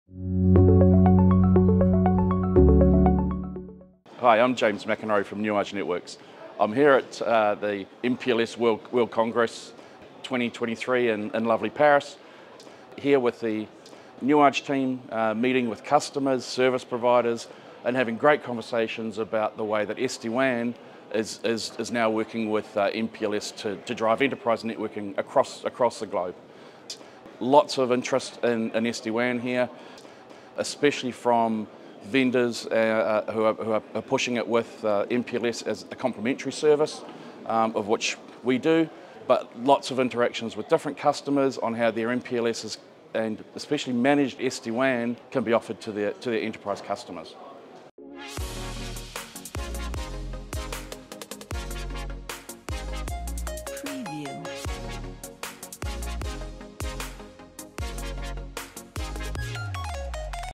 MPLSWC23: Maximizing Enterprise Networking with SD-WAN and MPLS Integration
*James McInroe* , Head of Marketing from *Nuage Networks* , explains how SD-WAN and MPLS work together to enhance enterprise networking. He covers how:
- SD-WAN is gaining interest as a complementary service to MPLS, driving global enterprise networking
- Vendors are exploring ways to offer managed SD-WAN alongside MPLS for enterprise customers
- The integration of SD-WAN and MPLS can provide more efficient and flexible networking solutions